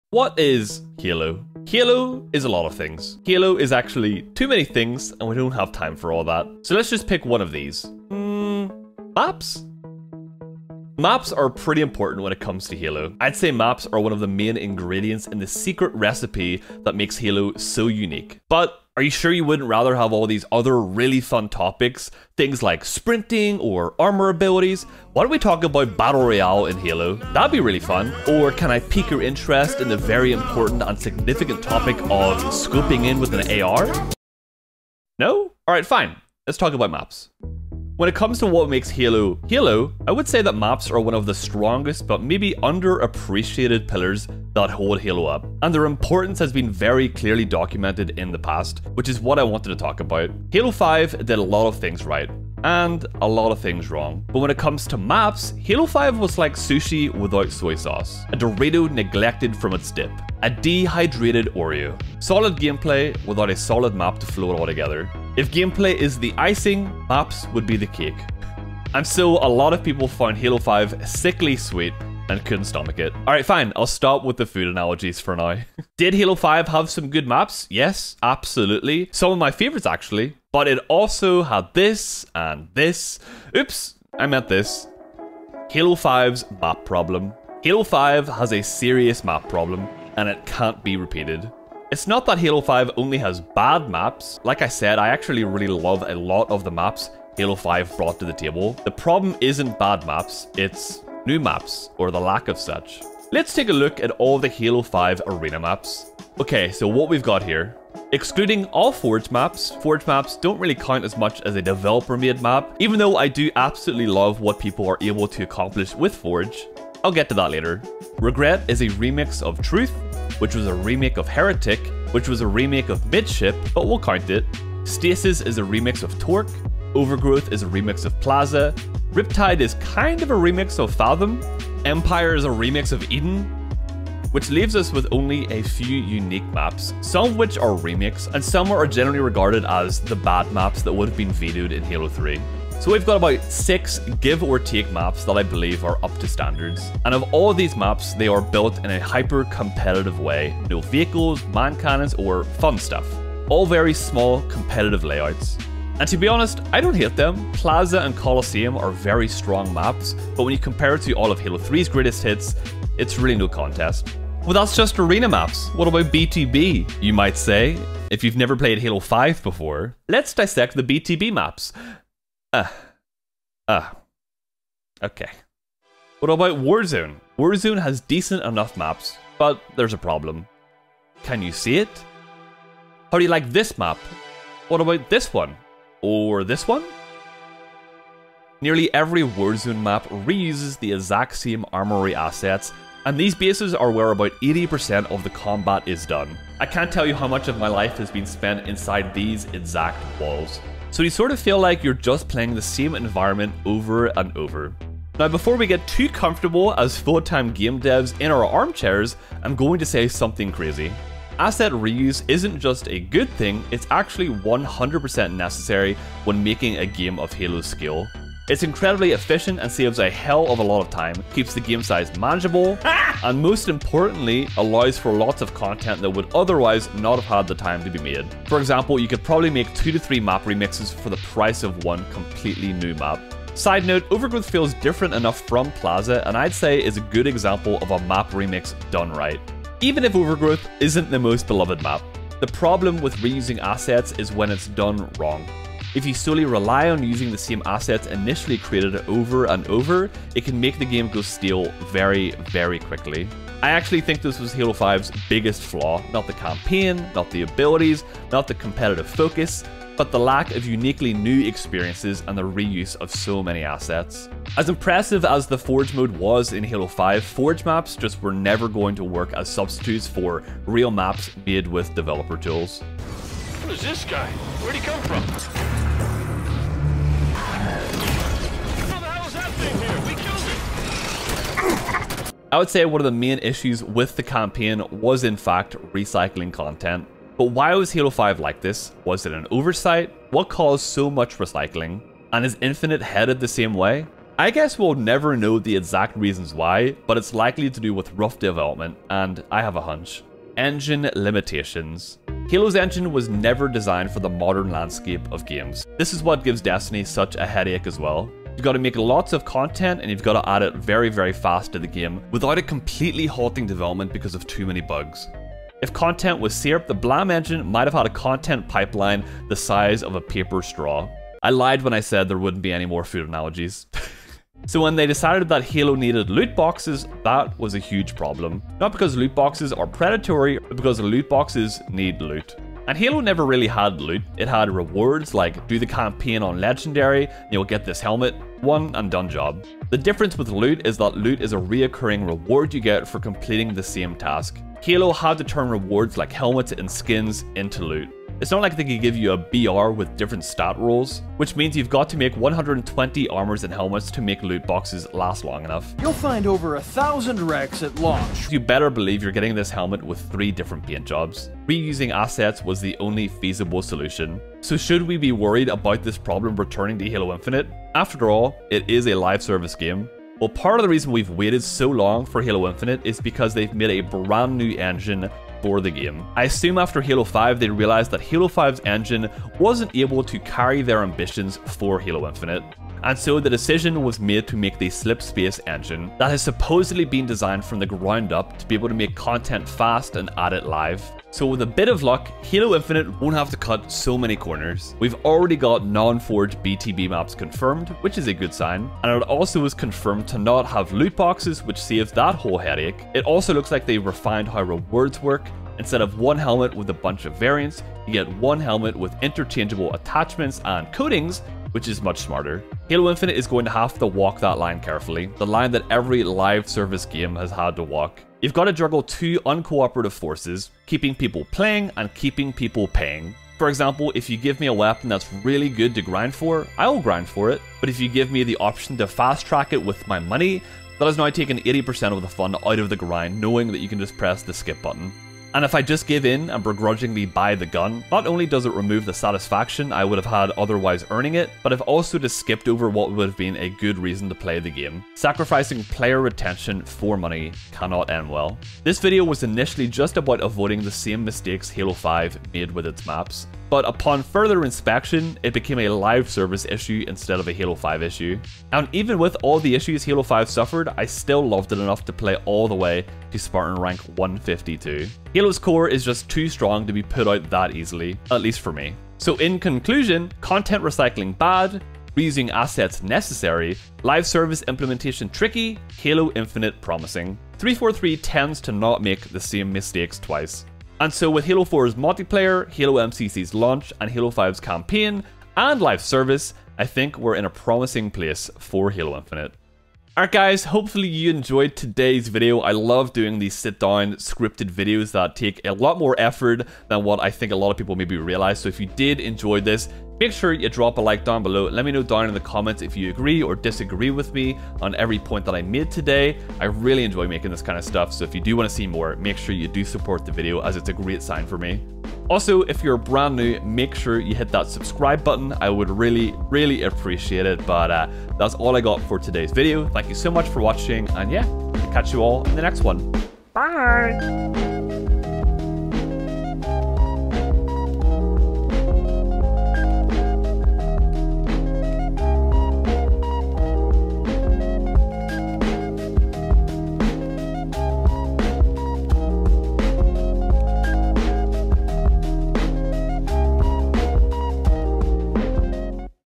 Halo Infinite CANNOT Repeat This Mistake...
In todays video we are are talking about one of the major issues that has flown under the radar. and how Halo Infinite is dealing with that in the next halo infinite technical preview BTB flight.